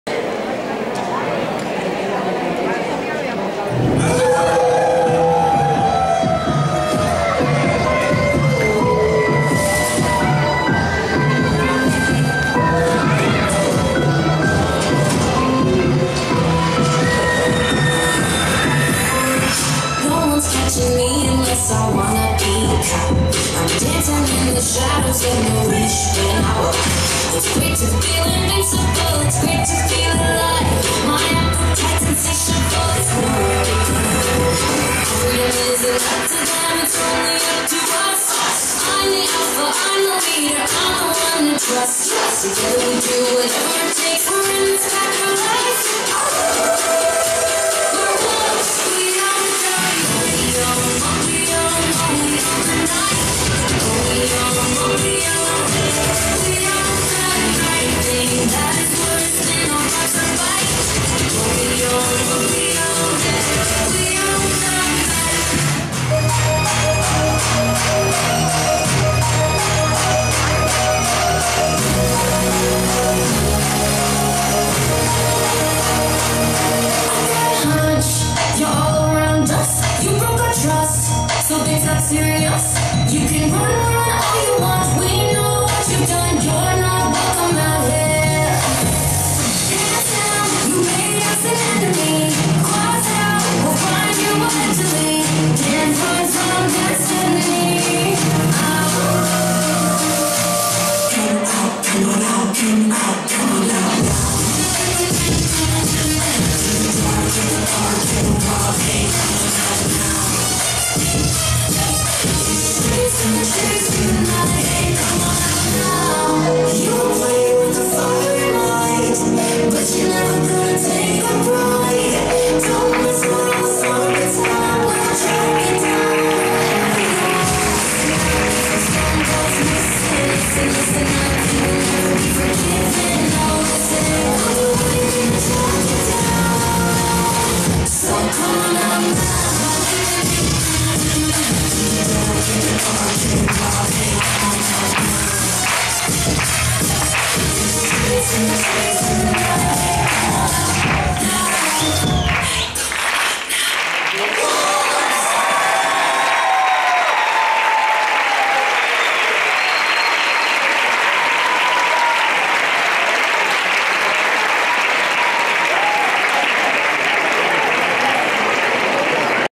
Funky Hip-Hop - Fin de curso 2023 - Lobitos
Actuación del grupo de FUNKY HIP-HOP en la exhibición de fin de curso de la academia. 10/07/2023